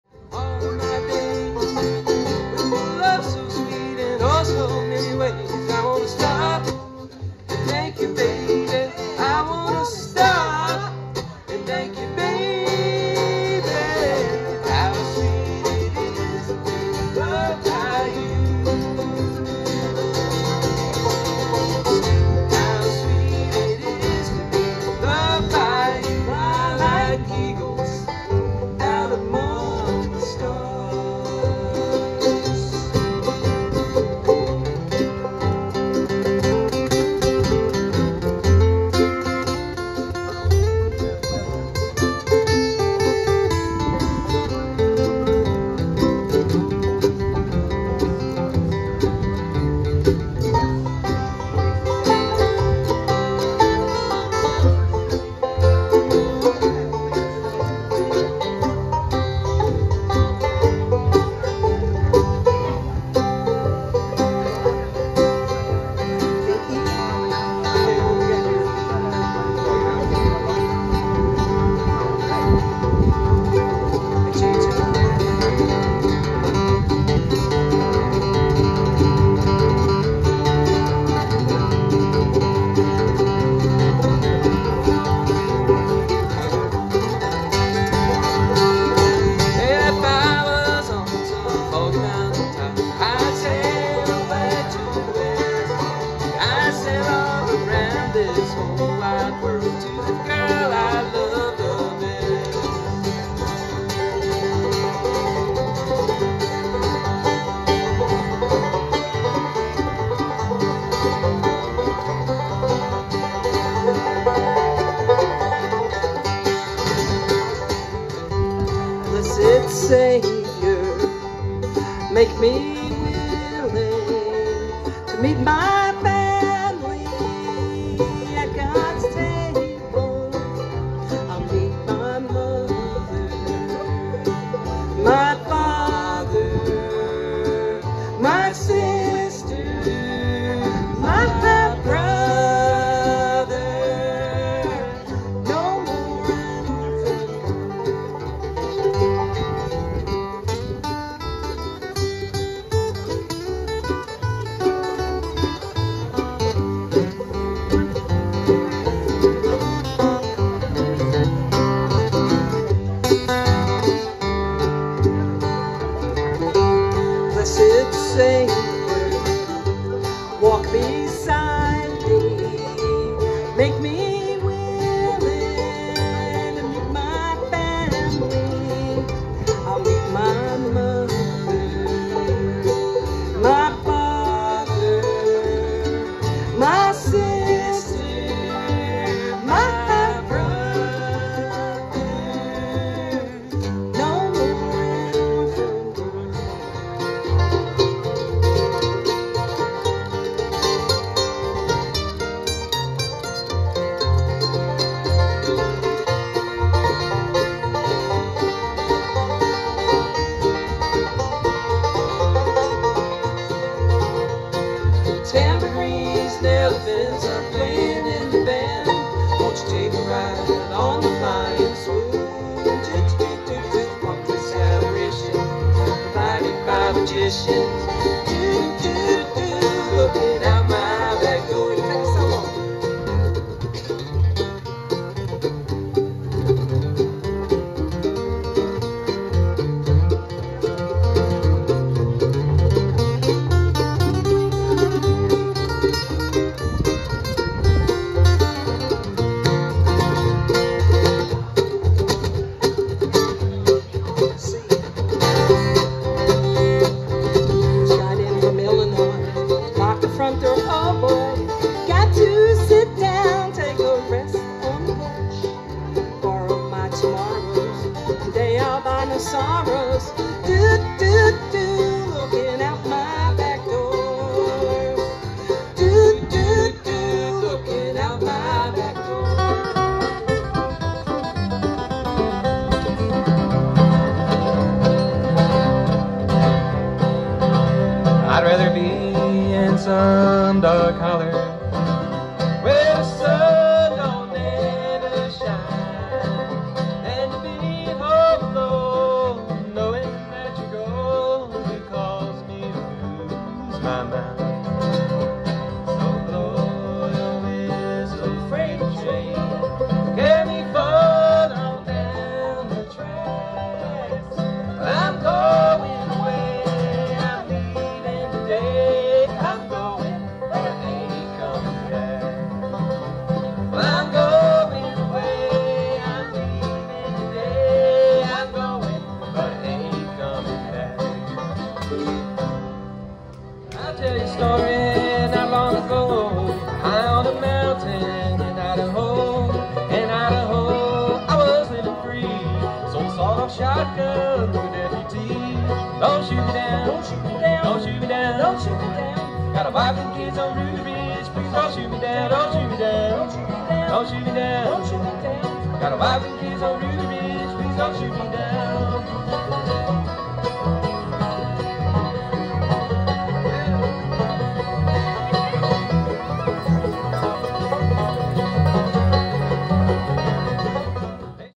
Keswick Vineyards Labor Day Sept 4 2023
WaterFalls Bluegrass Band with special guest Paul Fincham on banjo.
Same line-up is hitting the Keswick stage July 4 2024 from noon-4pm for the Reds, Whites, and Bluegrass Celebration!